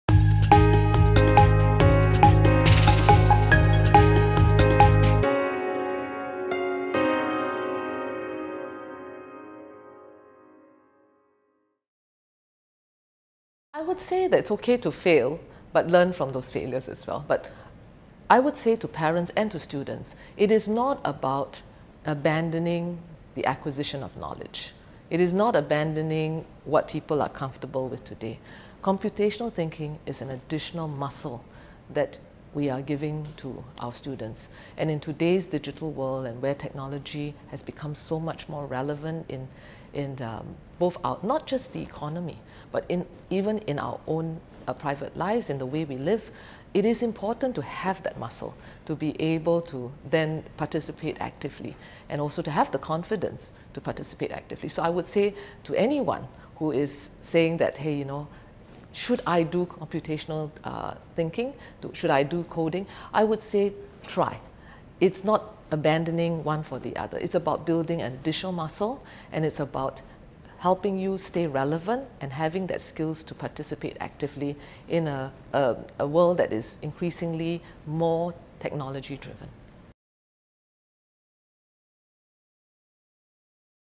Jessica Tan “Computational Thinking an Additional Muscle” | Perspectives | Channel NewsAsia
Jessica Tan Soon Neo, Managing Director of Microsoft Singapore: Computational thinking is not about abandoning the acquisition of knowledge. It’s about building that additional muscle that helps you stay relevant.